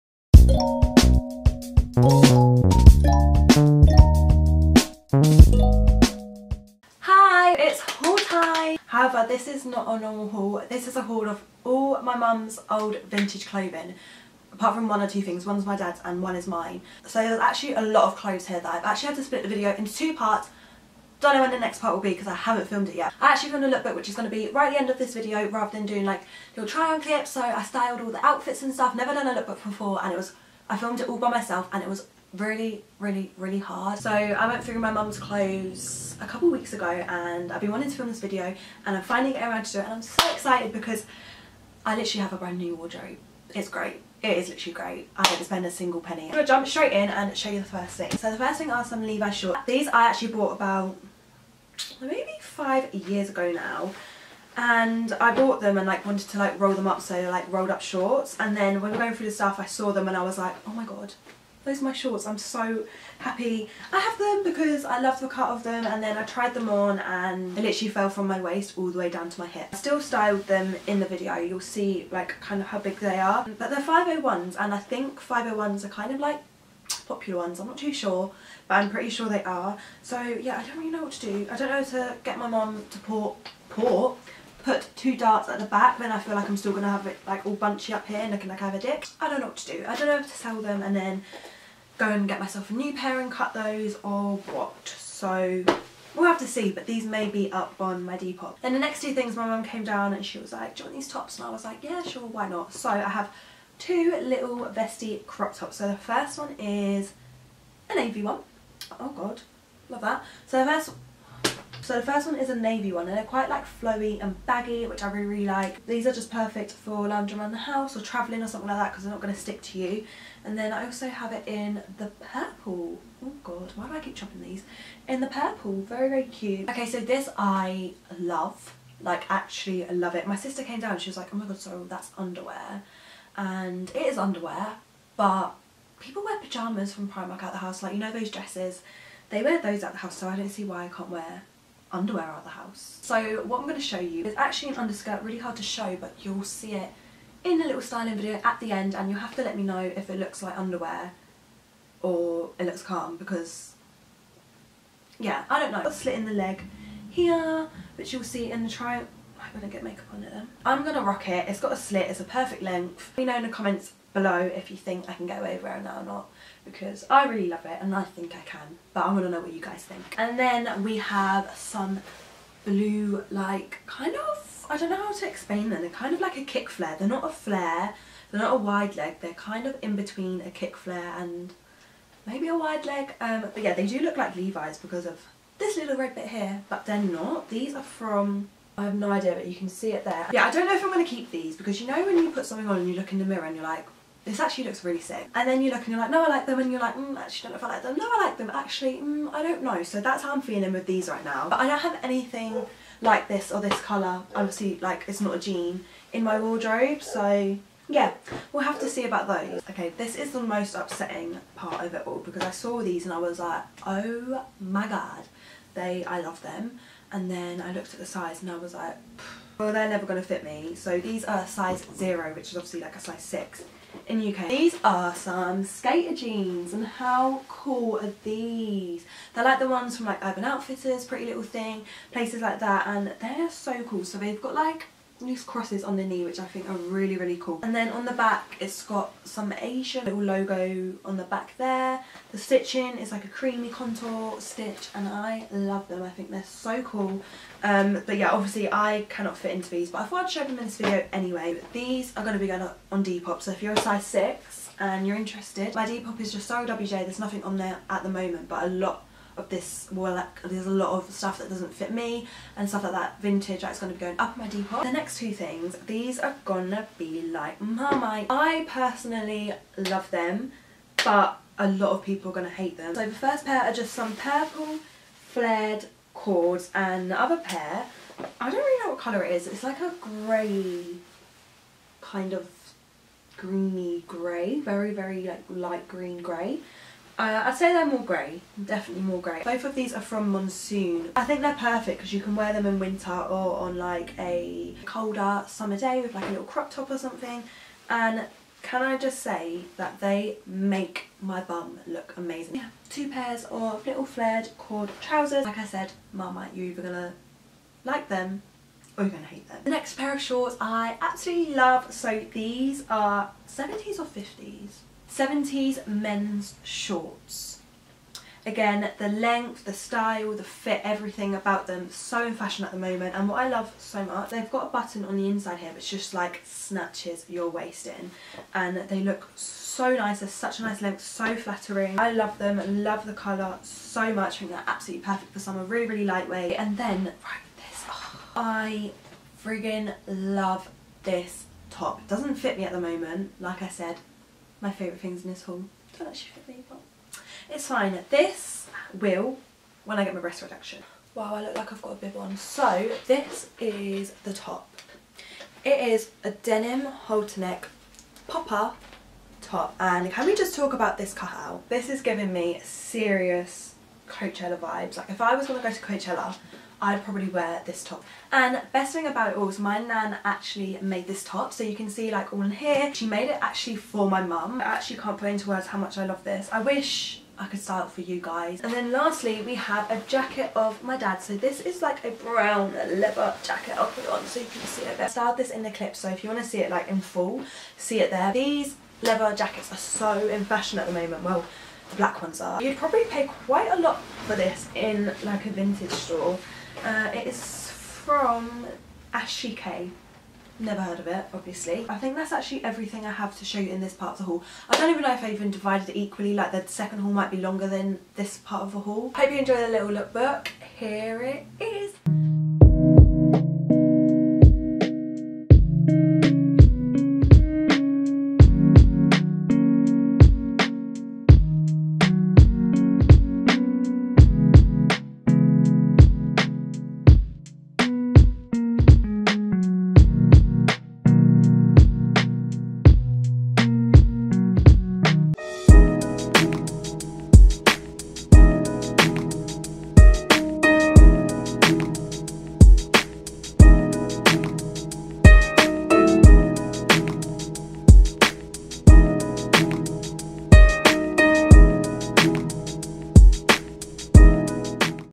A VINTAGE CLOTHING HAUL & LOOKBOOK!
Hiya loves! Today I have a vintage haul and lookbook of all my mums vintage clothing which is now mine hehe. I also did a little look book at the end so hope you enjoy xx